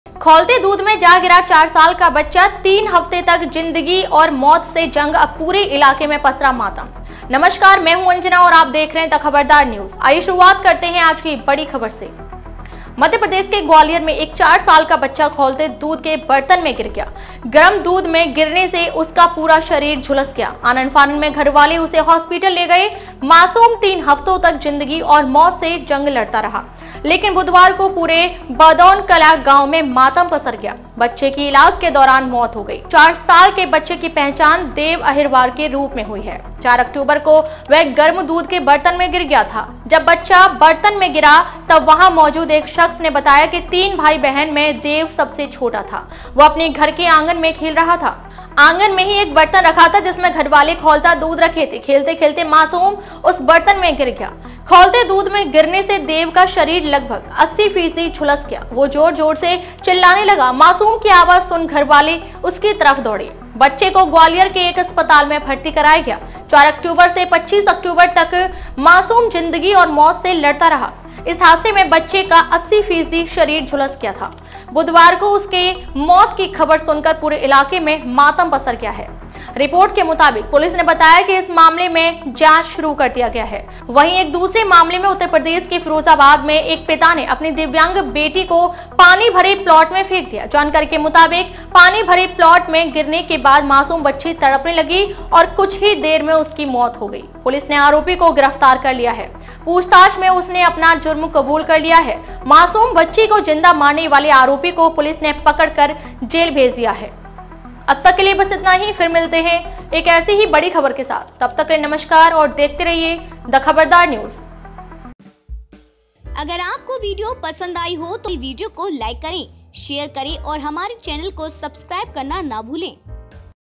GWALIOR News: खौलते दूध में जा गिरा 4 साल का बच्चा, हुई मौत । Child Death Accident case in Rewa.
GWALIOR News: खौलते दूध में जा गिरा 4 साल का बच्चा.